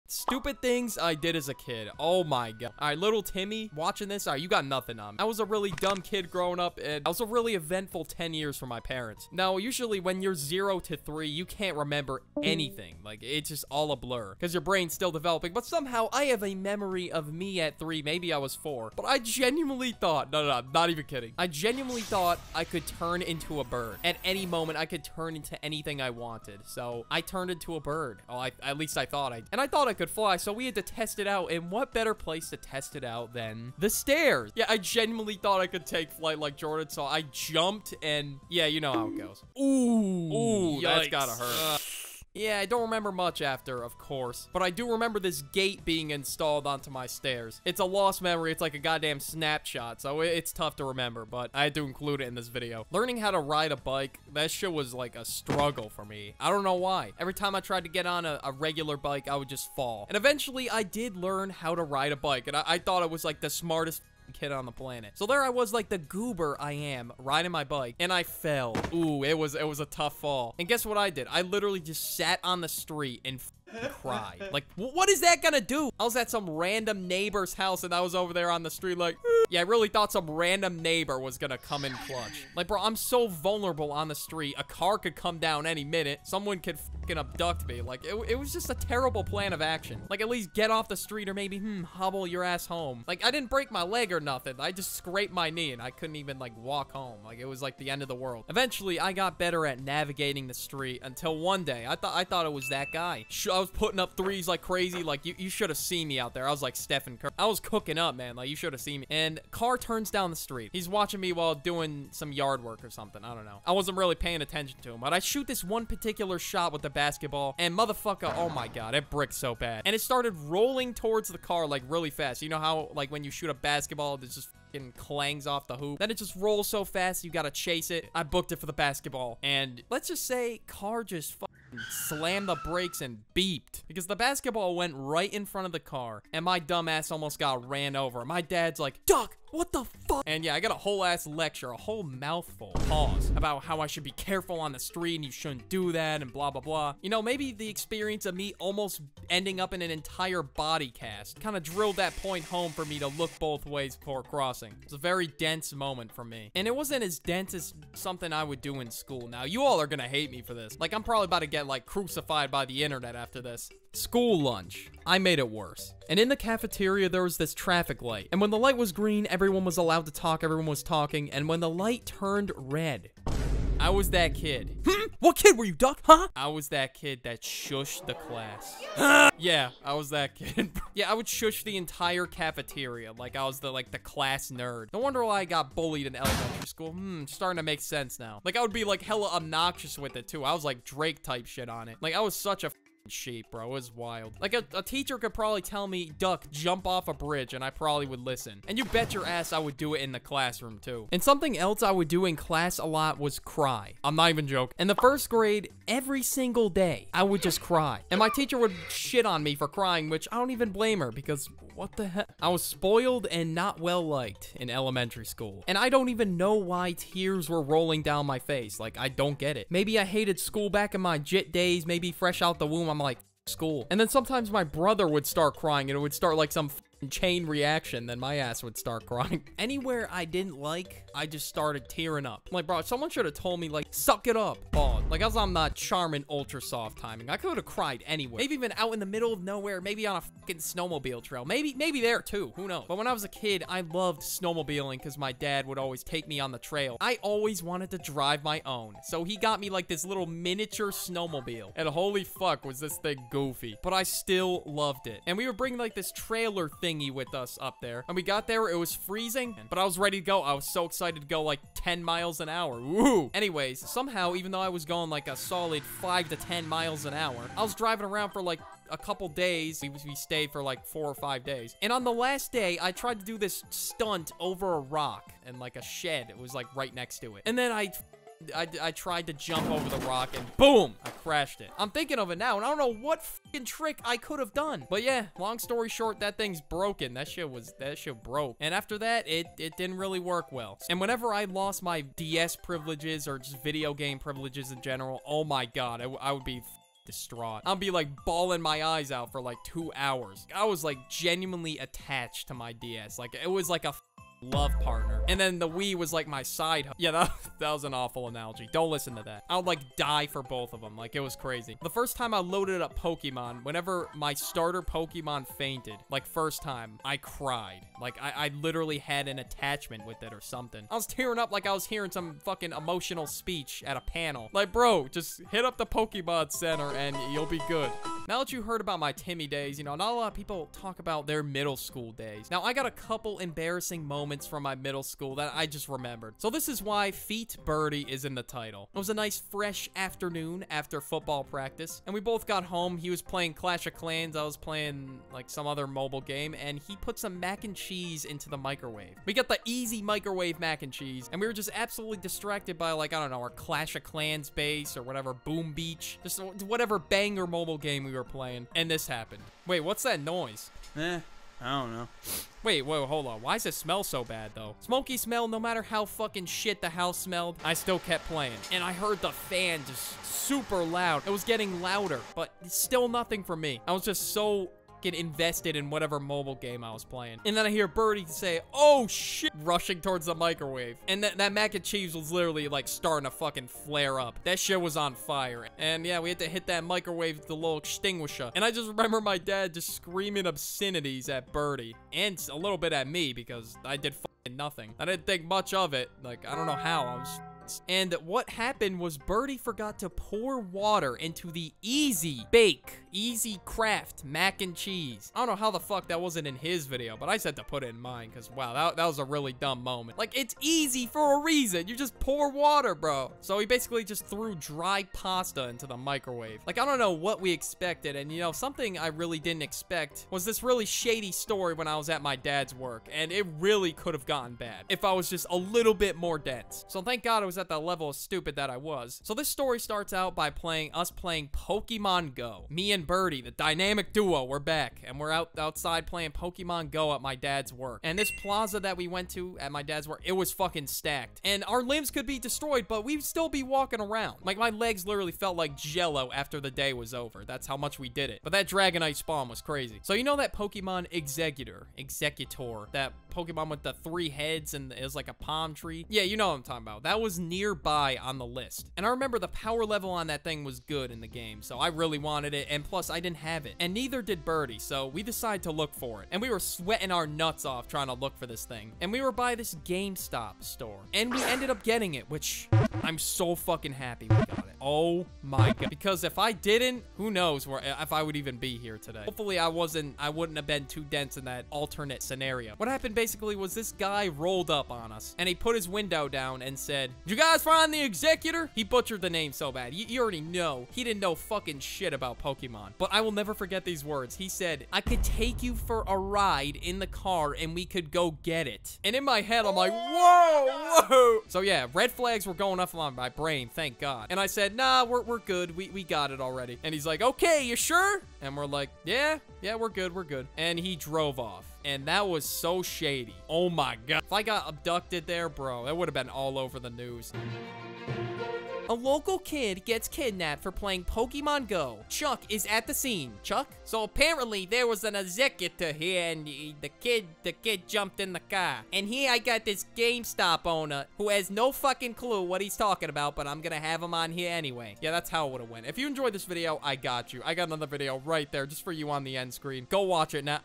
Stupid Things I Did As A Kid (Ft Burdie)
Stupid Things I Did As A Kid (Ft Burdie) today I talk about stupid things I did as a kid in school and out of school. These things we all did as kids maybe.. this video is with burdie another animation story youtuber